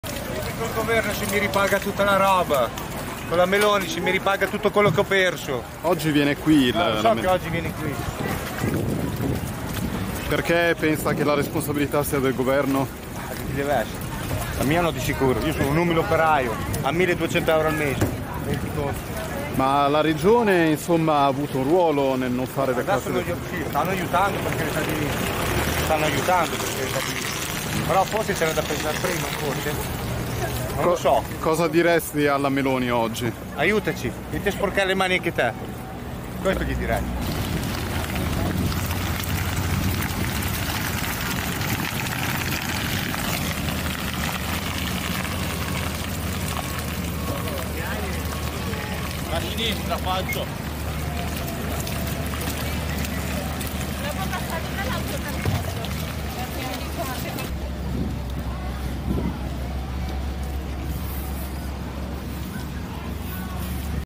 Forlì, un cittadino impegnato a ripulire la città dopo l'alluvione: "Chiedete a Meloni se mi ripaga"
"Chiedete al governo e alla Meloni se mi ripagano di tutto quello che ho perso". Queste le parole di un cittadino di Forlì impegnato nello spalare il fango insieme a molti altri residenti e volontari venuti da fuori per aiutare. "Cosa direi alla Meloni oggi? Aiutaci, vieni a sporcarti le mani anche te".